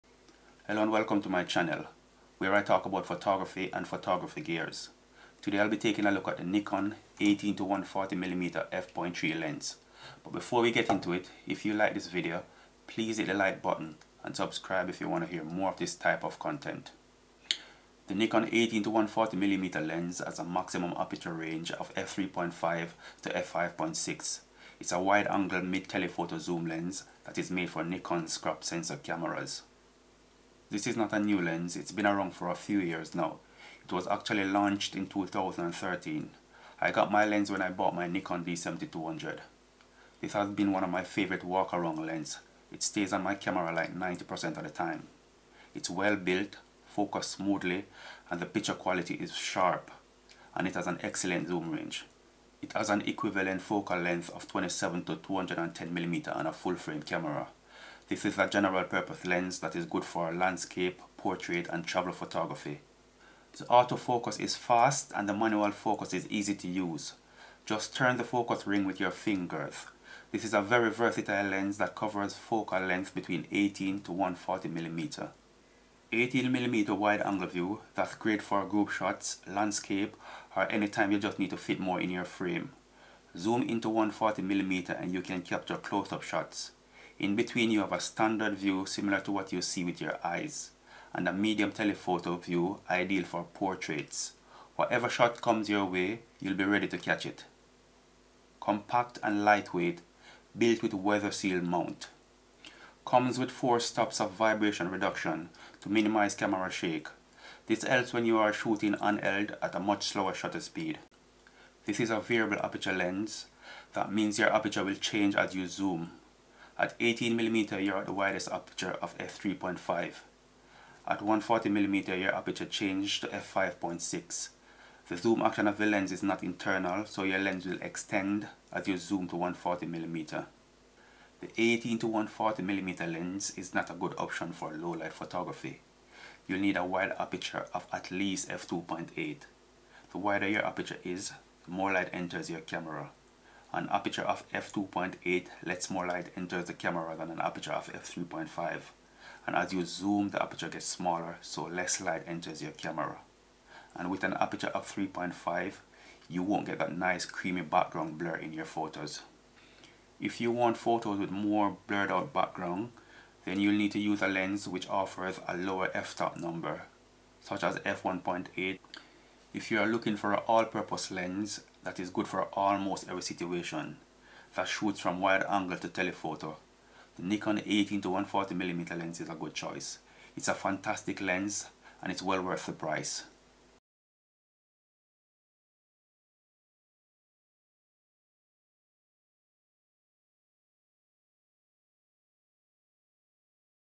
Nikon DX VR 18-140mm f3.5-56G ED, best lens for travel.
Nikon DX VR 18-140mm compact and lightweight , makes the perfect travel lens.